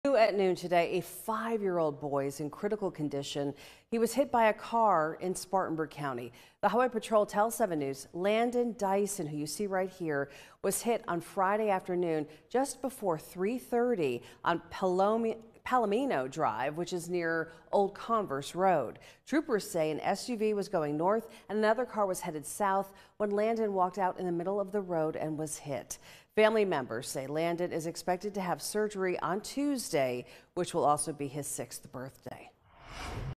5-year-old in critical condition after being hit by car in Spartanburg Co.
5-year-old in critical condition after being hit by car in Spartanburg Co.